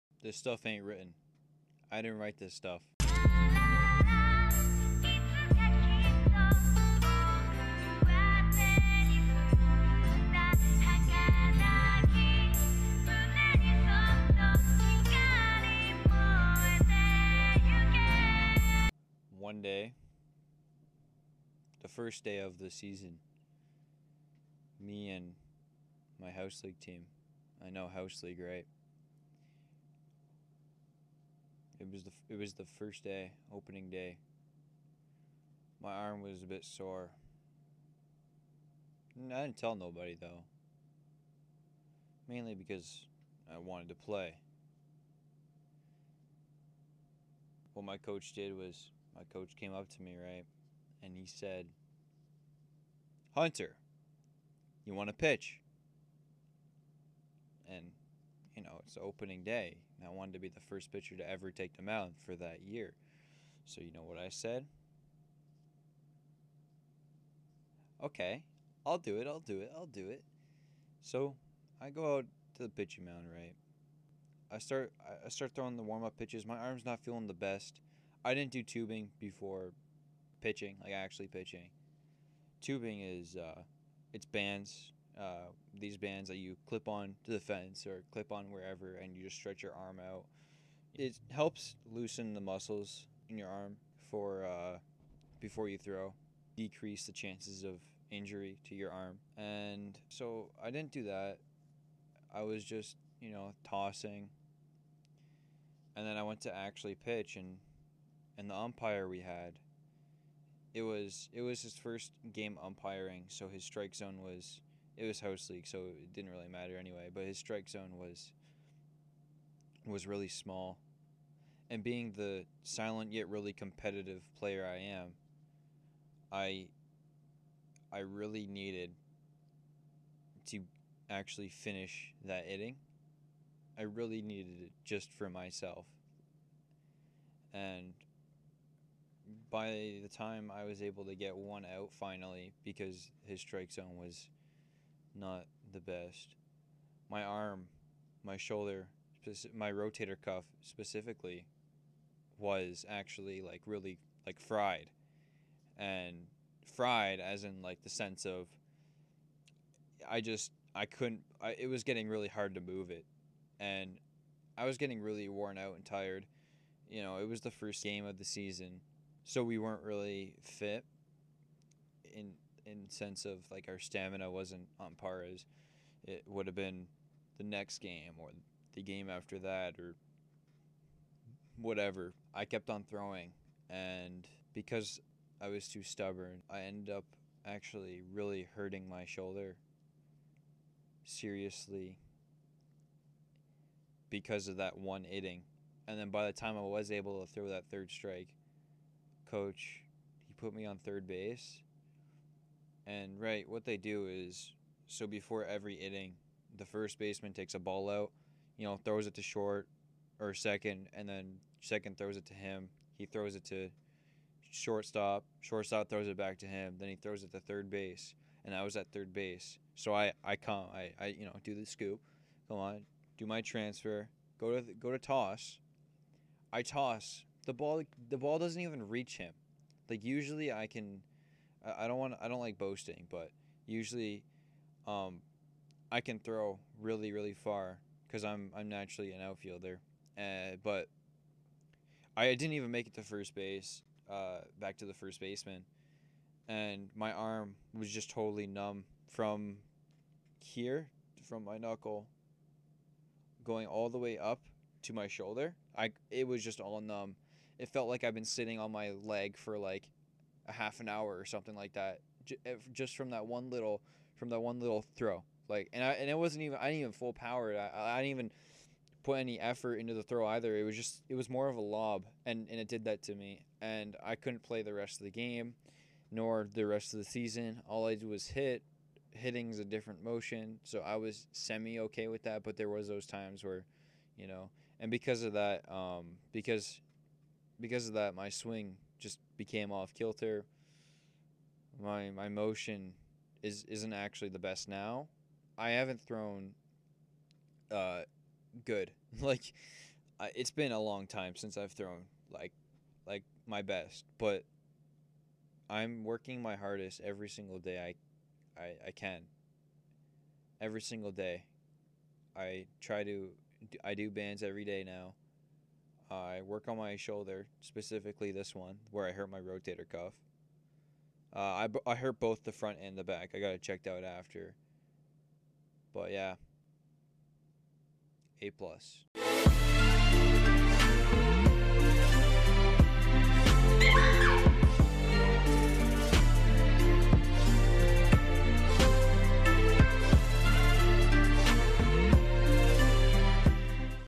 The Time I hurt My Rotator Cuff
I tell you about the time I hurt my rotator cuff during baseball
(context I needed to do it for school so I thought why not tell everyone)